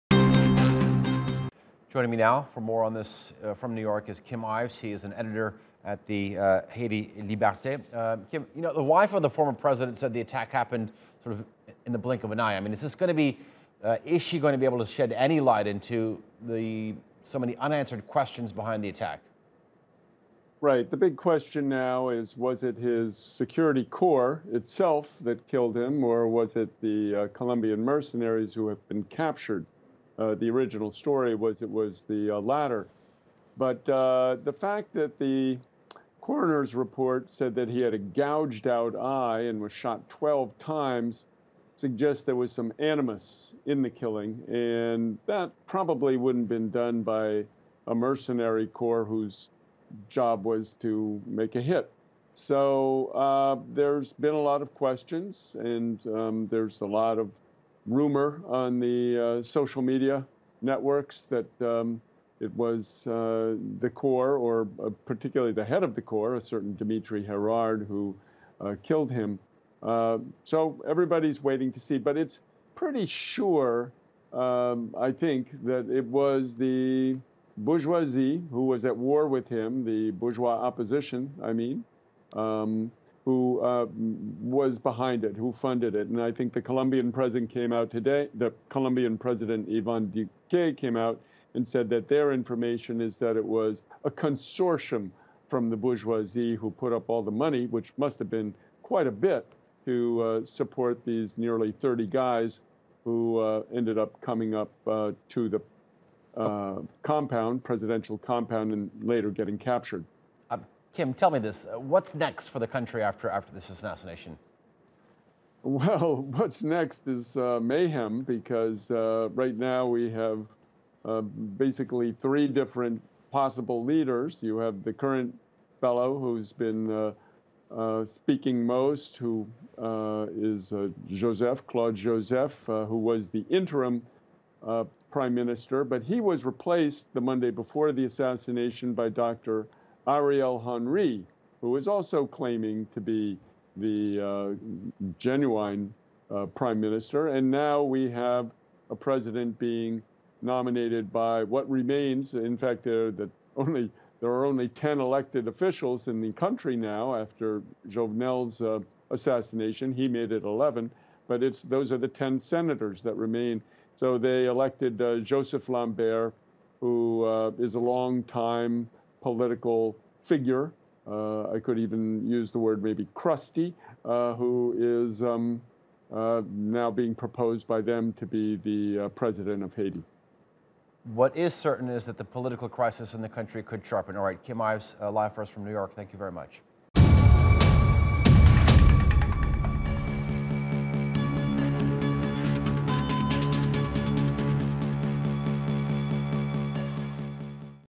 What is next for Haiti after the assassination of Jovenel Moise?
We speak to journalist Kim Ives about the assassination of President Jovenel Moise and what is next for Haiti after the killing of the country’s leader.